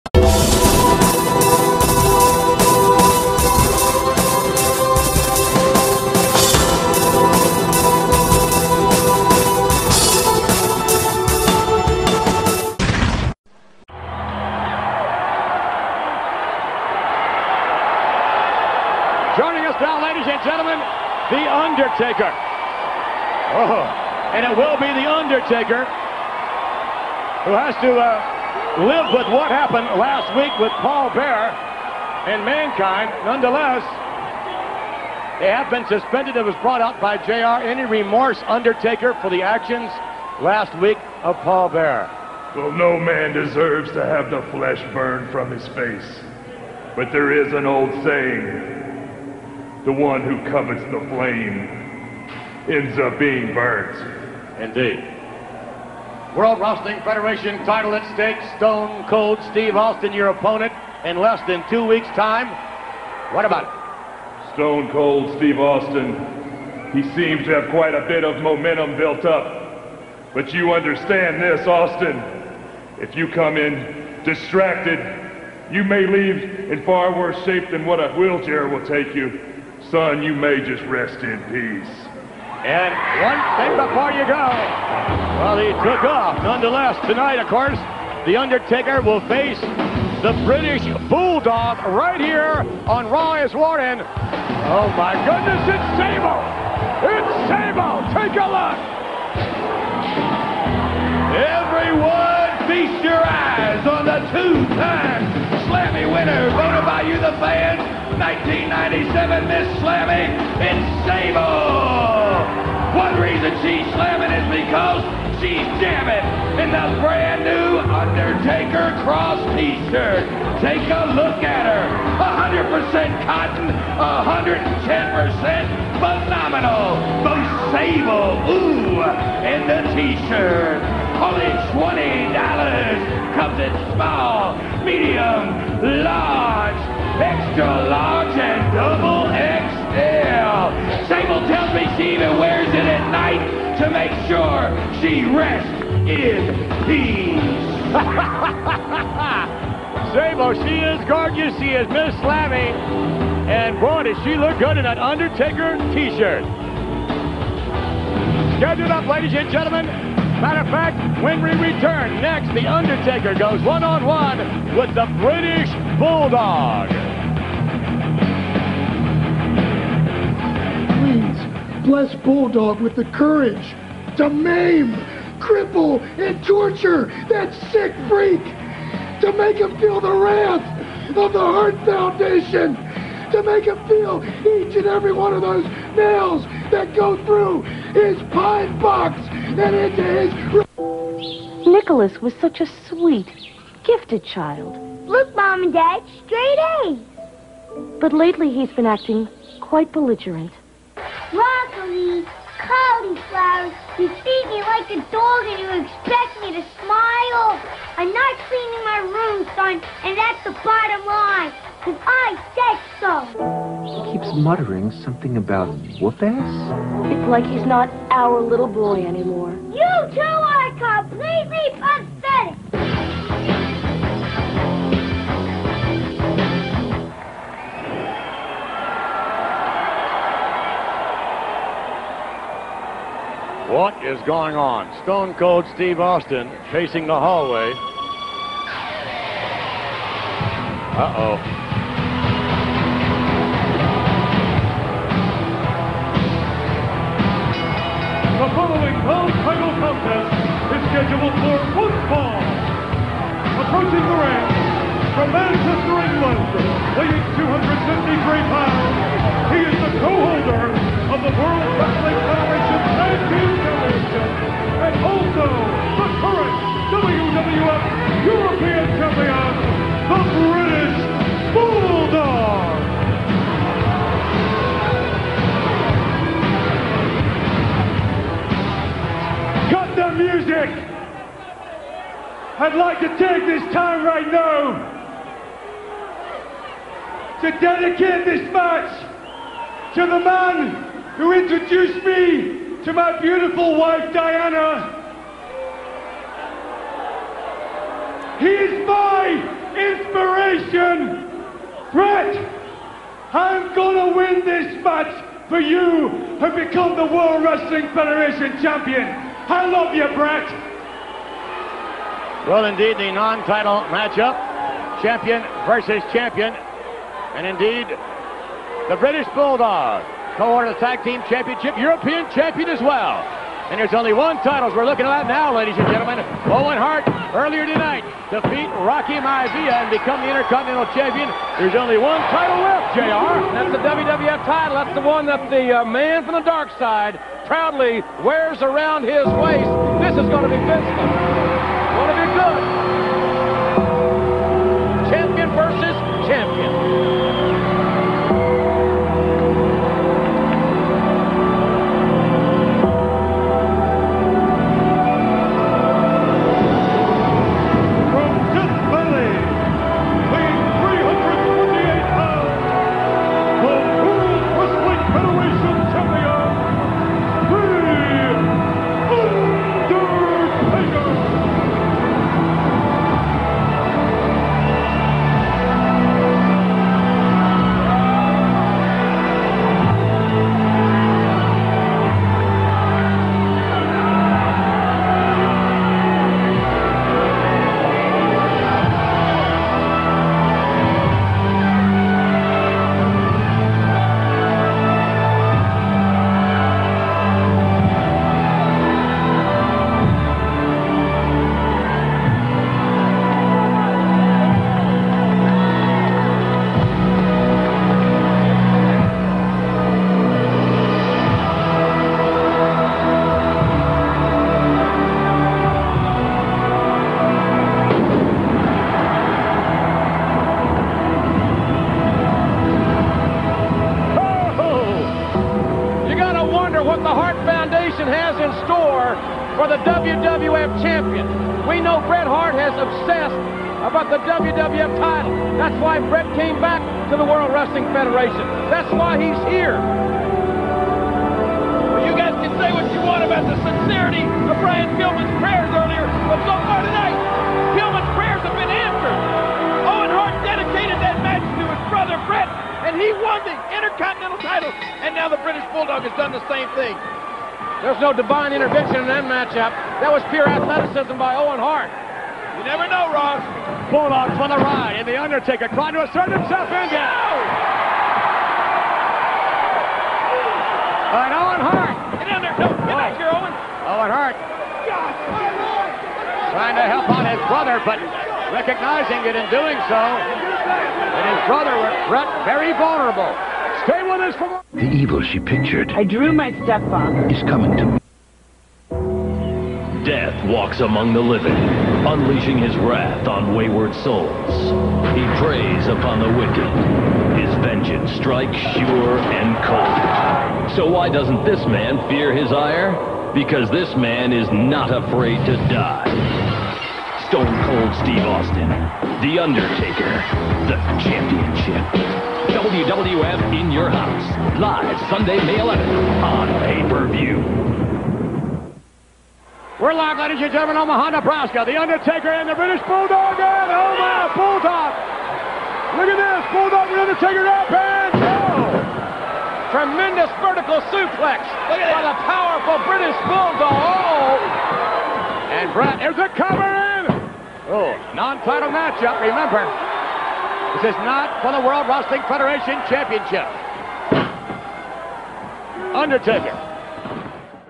Undertaker - 4/28/97 1/2
Taker cuts a promo for his match against austin for the wwf title + taker vs bulldog part 1.